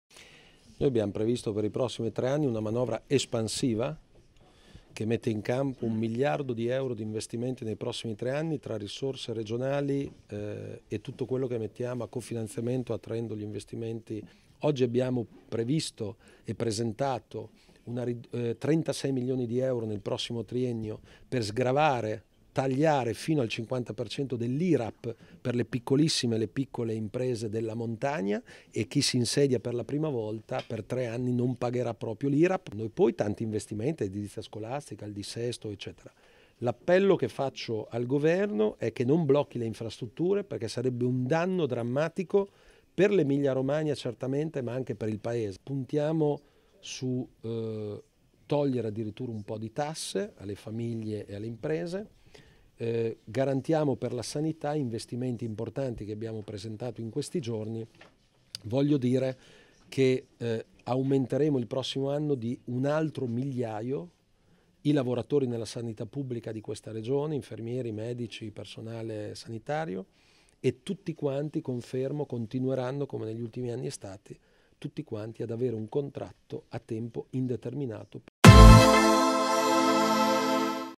Presentato il bilancio 2019 della Regione Emilia-Romagna
Conti in ordine, meno tasse per cittadini e imprese, pieno utilizzo fondi per investimenti da 1 miliardo. Sviluppo e lavoro, ambiente e territorio, montagna, turismo, mobilità sostenibile.
L'intervista al presidente Stefano Bonaccini

Regione Emilia-Romagna
viale Aldo Moro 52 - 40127 Bologna